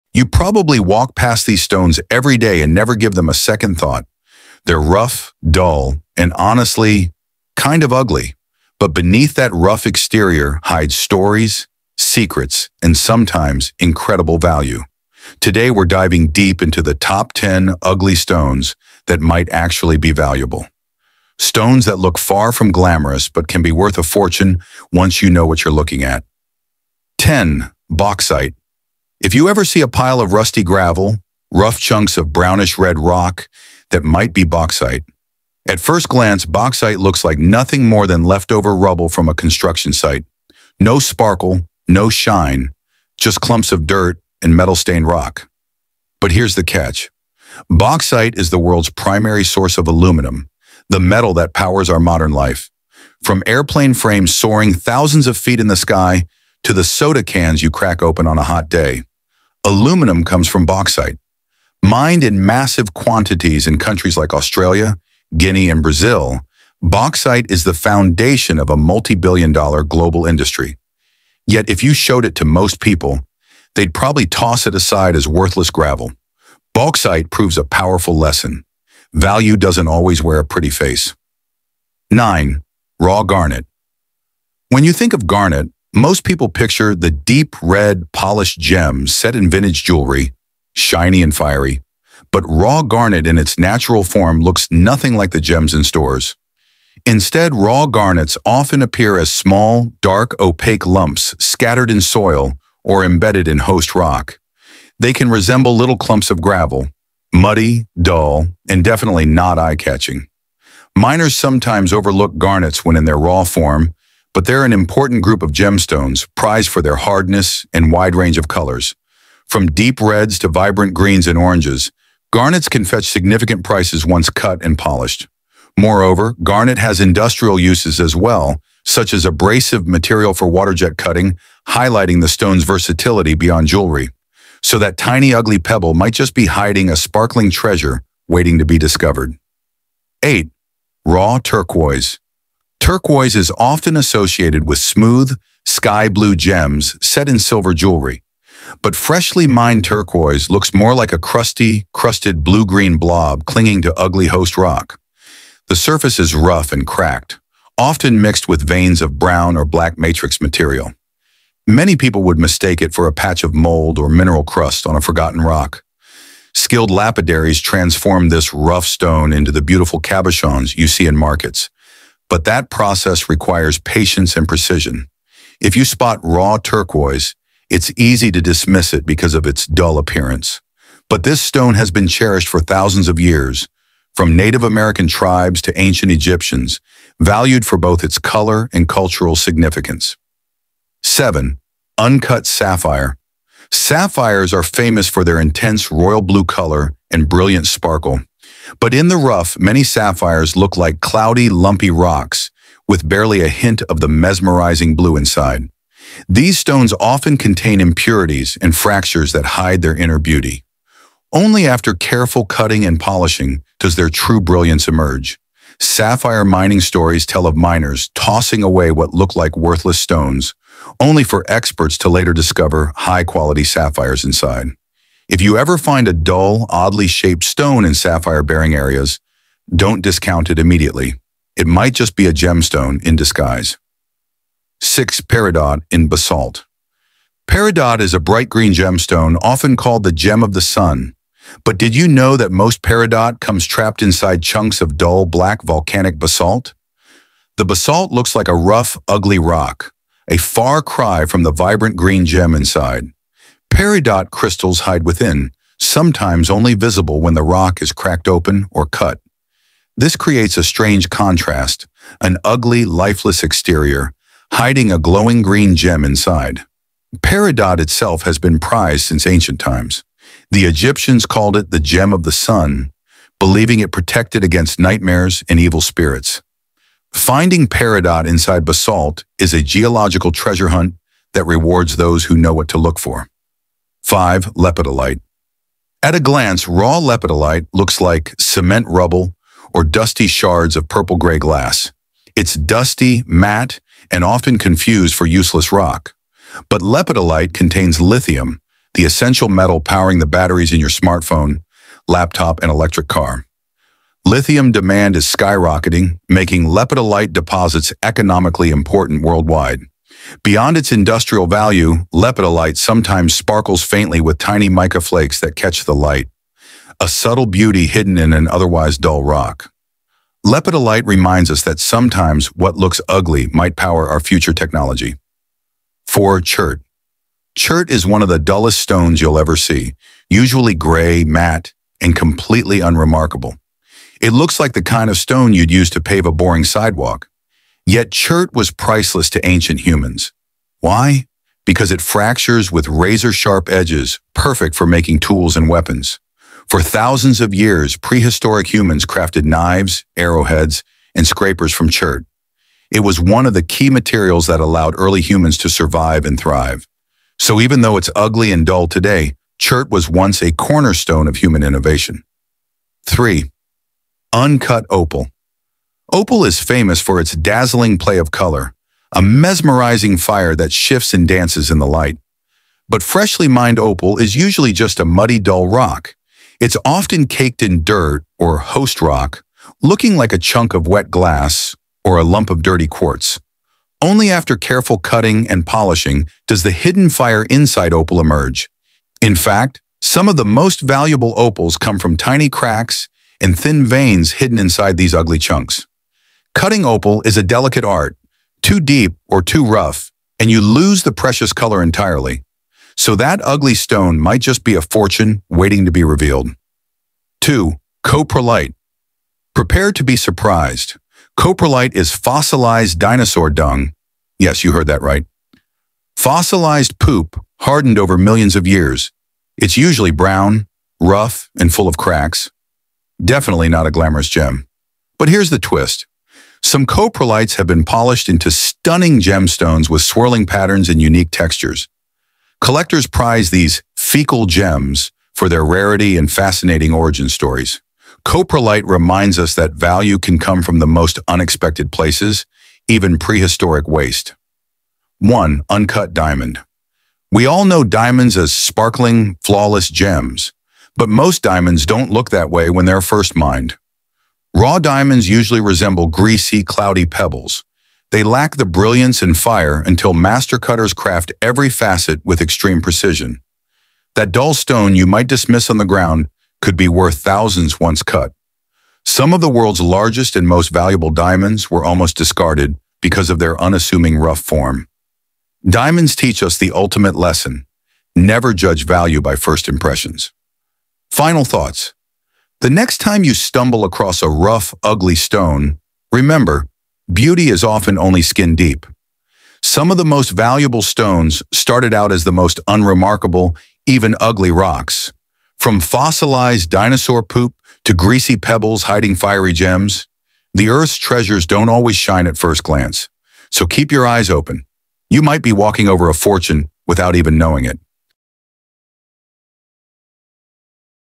You’ve Walked Past These Ugly Stones Without Knowing Their Value
From raw garnets and crusty turquoise to fossilized dung polished into gems, the world is full of stones that don’t look like much—until you take a closer look. This video reveals 10 ugly stones that might actually be worth more than gold once transformed by nature and human skill.

We’ll explore how these unassuming rocks form, where to find them, and how their value skyrockets once cut, polished, or refined. Whether you’re into rockhounding, gem hunting, or simply love surprising nature facts, you’ll never look at a boring stone the same way again.

Watch until the end—you won’t believe what the number one ugly stone turns into!

🎙️ This video uses a professional AI-generated voice (Brian by ElevenLabs) for narration. No real individuals are mimicked or altered. This content is 100% original and educational, disclosed in accordance with YouTube’s synthetic content policy.